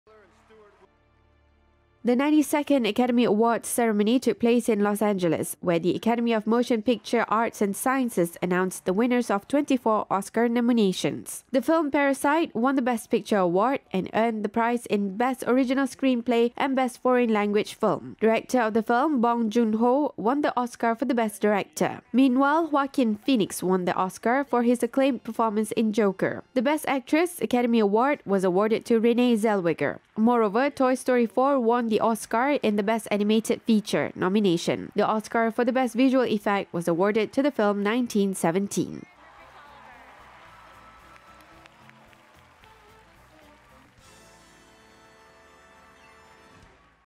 Oscars 2020 Took Place in Los Angeles
The 92nd Academy Awards ceremony took place in Los Angeles, where the Academy of Motion Picture Arts and Sciences announced the winners of 24 Oscar nominations. The film Parasite won the Best Picture award and earned the prize in Best Original Screenplay and Best Foreign Language Film. The director of the film, Bong Joon Ho, won the Oscar for Best Director.